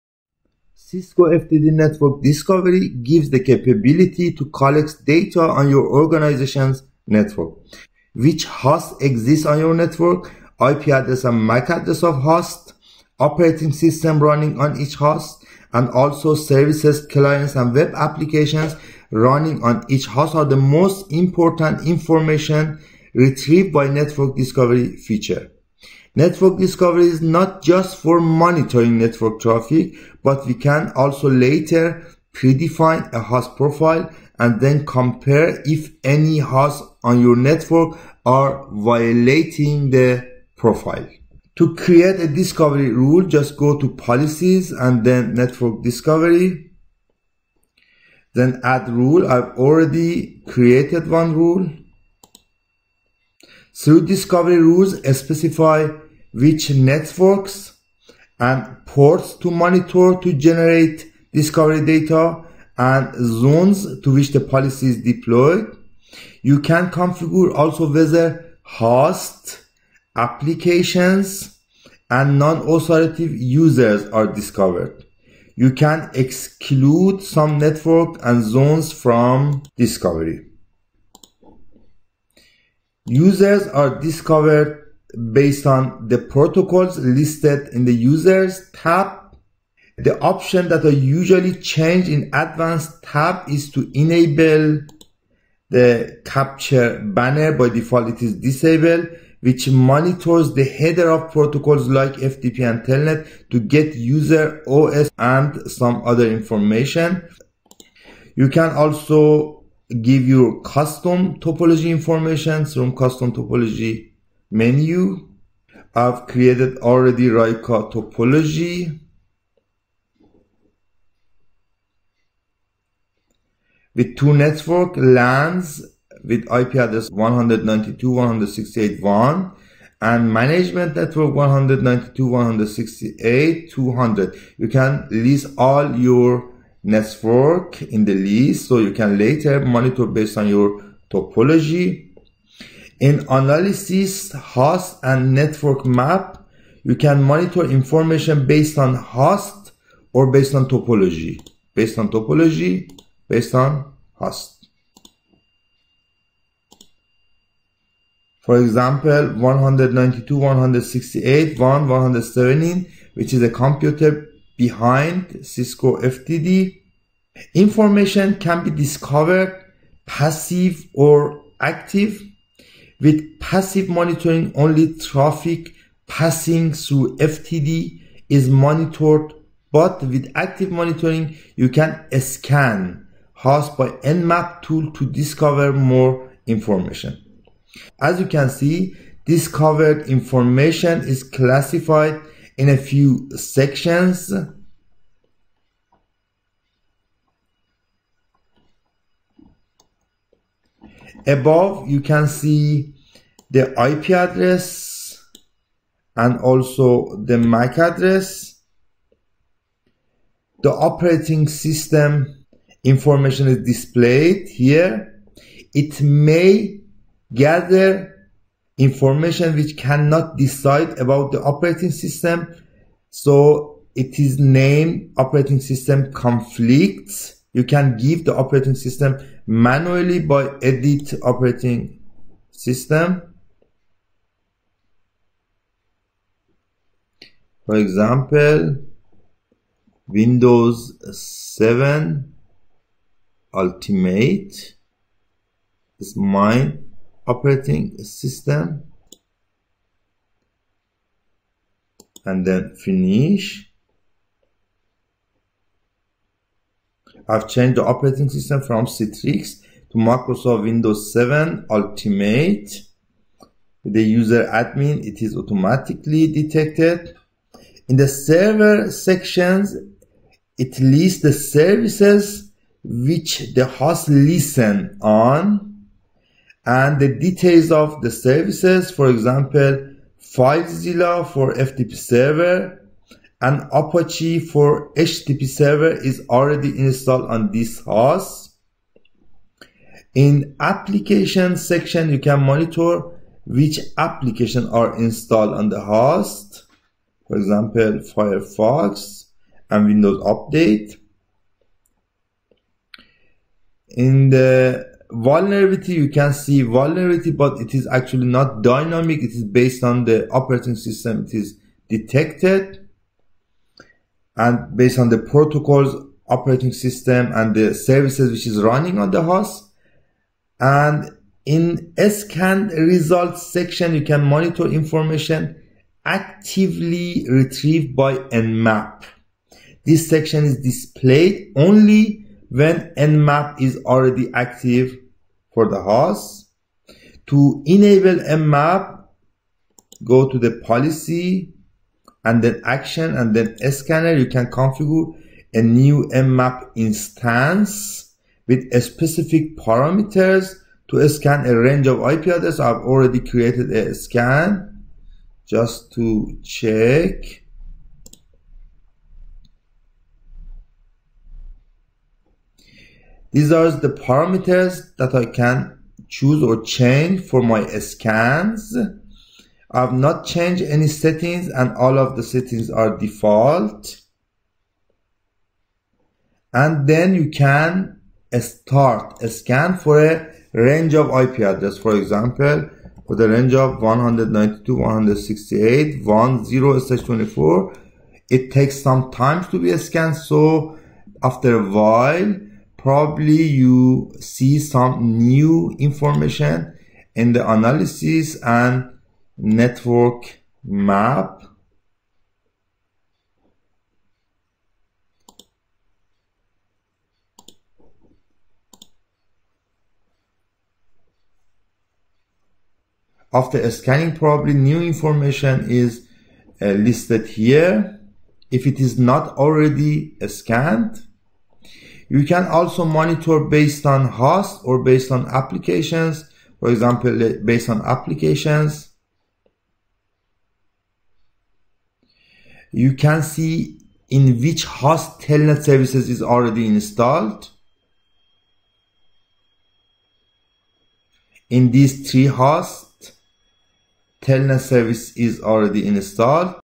20. Cisco FTD Network Discovery Policy
Cisco FTD Network Discovery gives the capability to collects data on your organization’s network.
Which hosts exists on your network? IP address and MAC address of hosts on your network, operating system running on each host and also clients and web applications running on each host are the most important information retrieved by  network discovery feature.
Network Discovery is not just for monitoring network traffic, but we can later predefine a host profile and then compare if any hosts on the network are violating the profile.